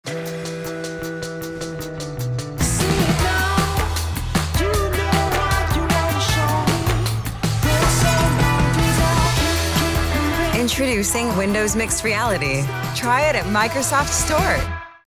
Introducing Windows Mixed Reality
Immerse yourself in a new reality. Escape to a world of your dreams with hundreds of exciting titles, and the fastest set-up time available. Try Windows Mixed Reality today.